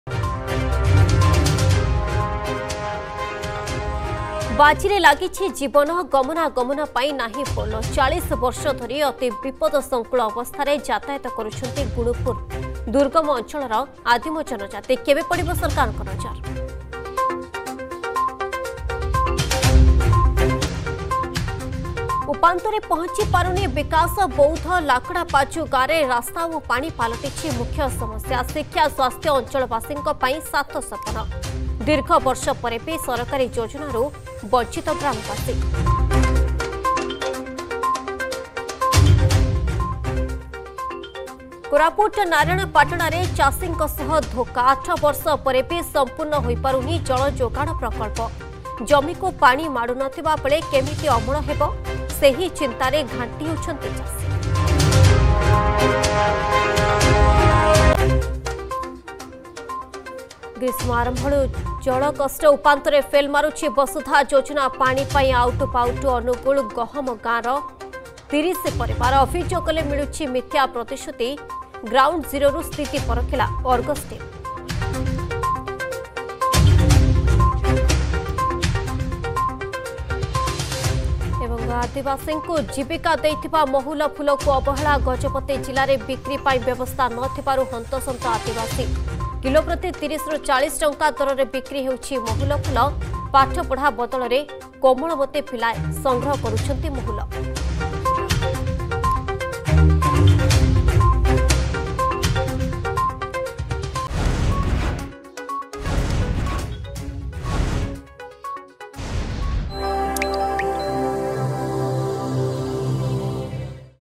Headlines 6.30 | 18 Mar 2022 | Argus News
+ ବାଜିରେ ଲାଗିଛି ଜୀବନ
+ଉପାନ୍ତରେ ପହଞ୍ଚି ପାରୁନି ବିକାଶ
+କୋରାପୁଟ ନାରାୟଣପାଟଣାରେ ଚାଷୀଙ୍କ ସହ ଧୋକା
+ ଗ୍ରୀଷ୍ମ ଆରମ୍ଭରୁ ଜଳକଷ୍ଟ
+ ଆଦିବାସୀଙ୍କୁ ଜୀବୀକା ଦେଇଥିବା ମହୁଲ ଫୁଲକୁ ଅବହେଳା